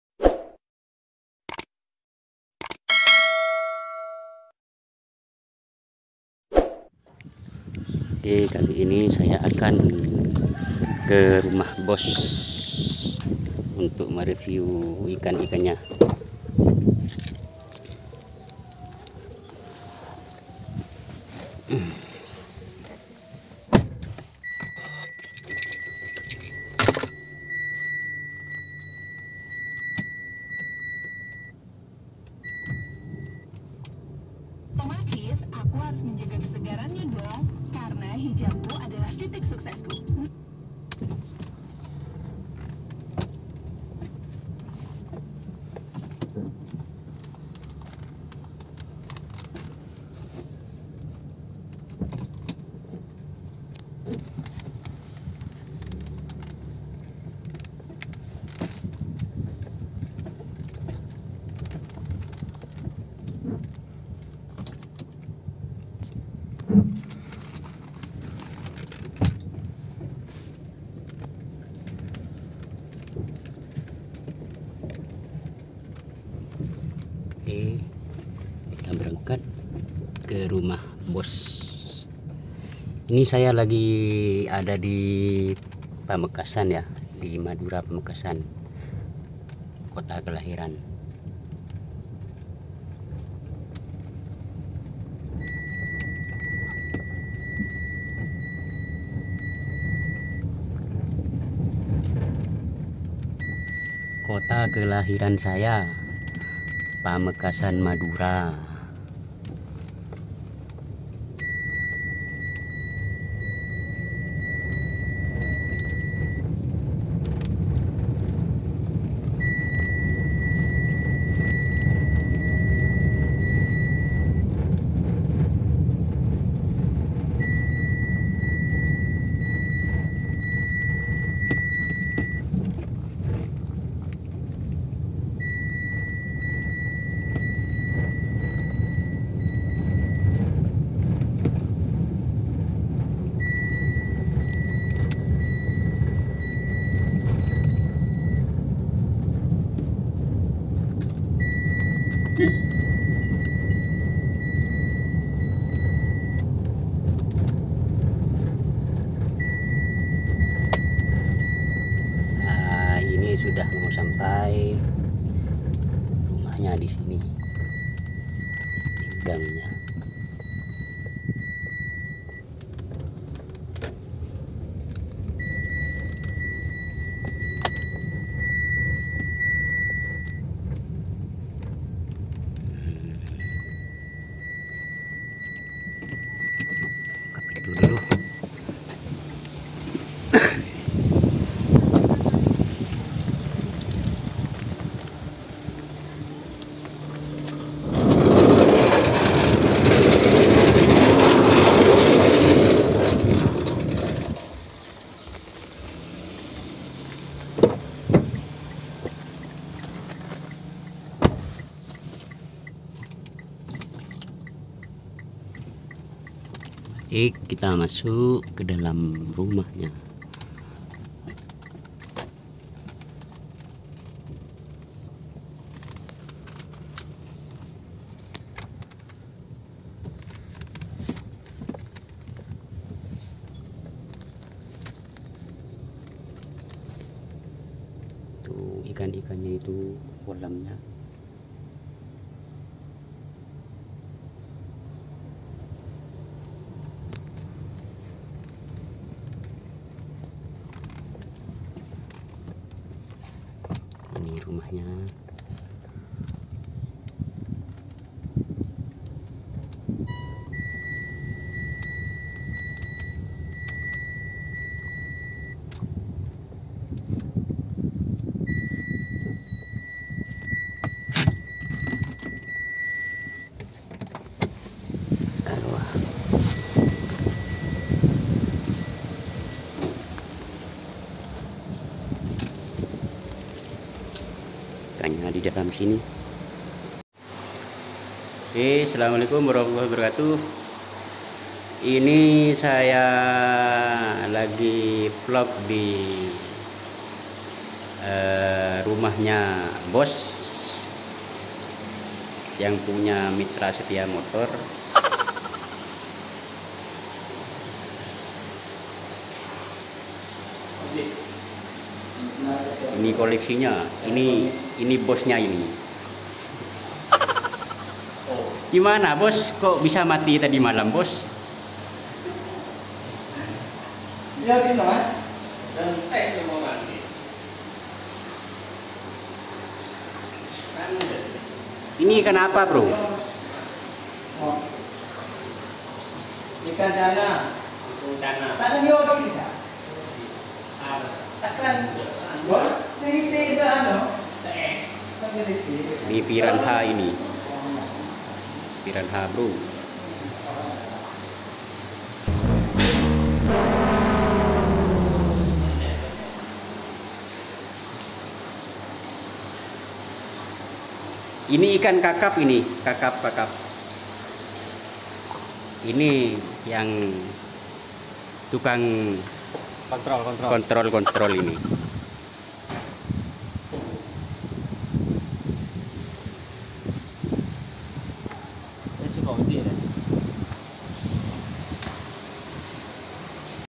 Ngereview koleksi ikan hias di rumah bos besar madura pamekasan !!!!
Terus pantau ngevlog bareng sy di kota pamekasan madura.. .
kali ini sy akan ngereview koleksi ikan hias bos besar pamekasan madura. Ok tonton sampai tuntas vidio ini sambil ambil kopinya dan duduk santai 😀😁😁😁😁.
Terimakasih atas kunjungan anda di channel ini.
salam sukses buat semuanya 👍🙏